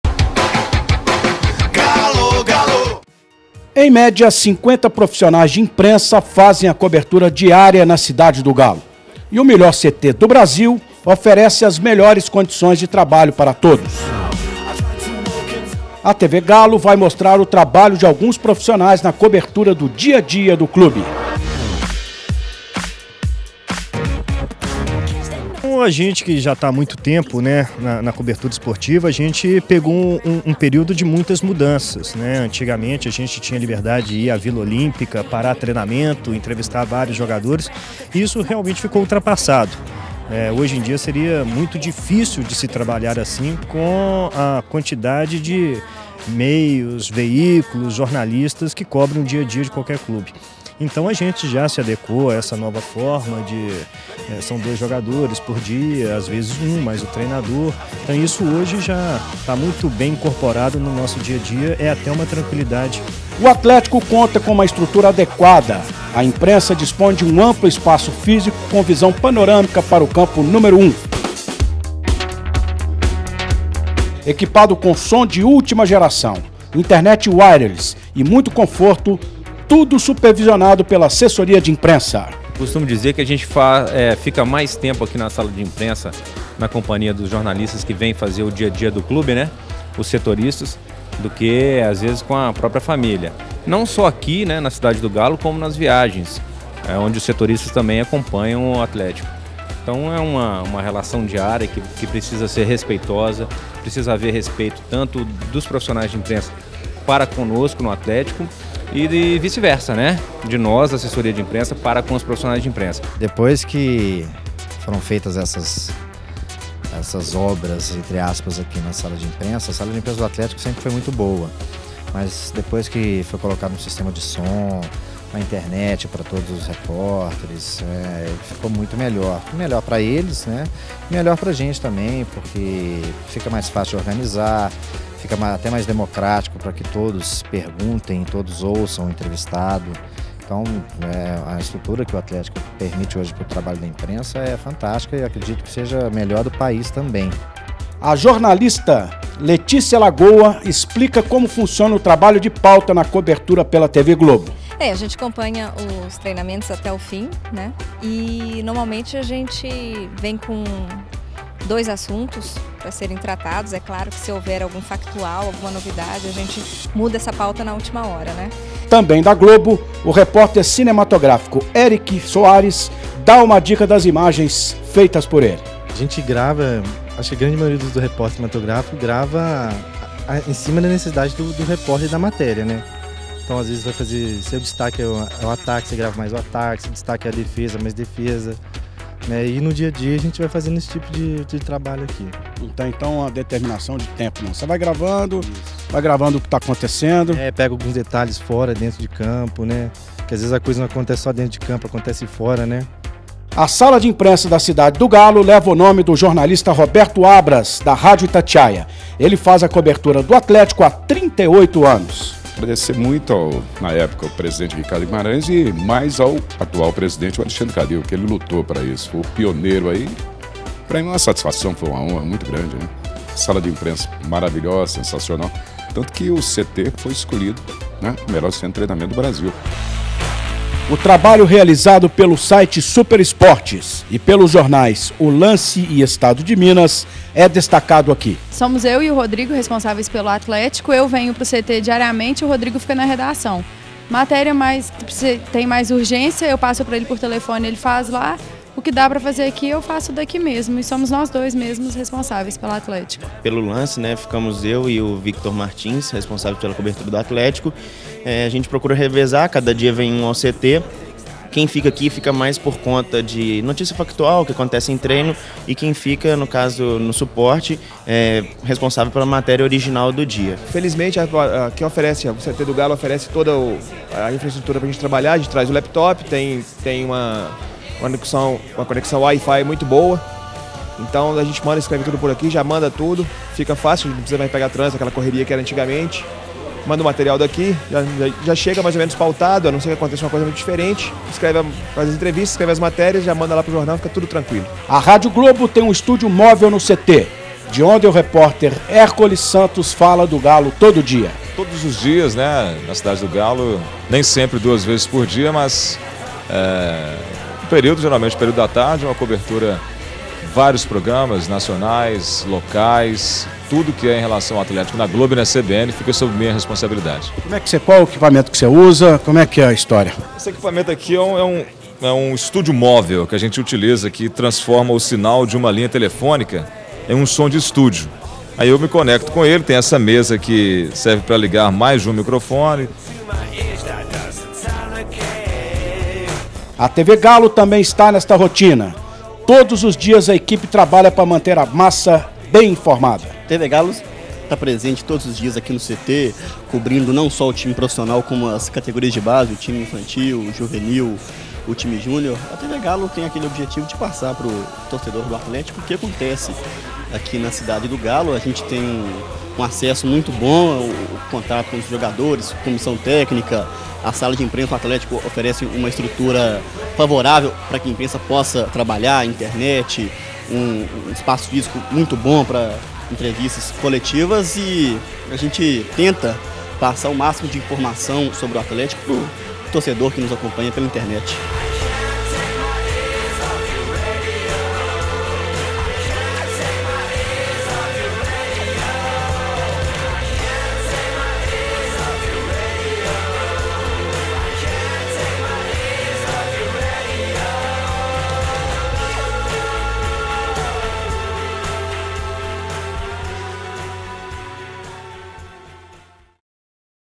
Sala de Imprensa
A TV Galo vai mostrar o trabalho de alguns profissionais na cobertura do dia a dia do clube.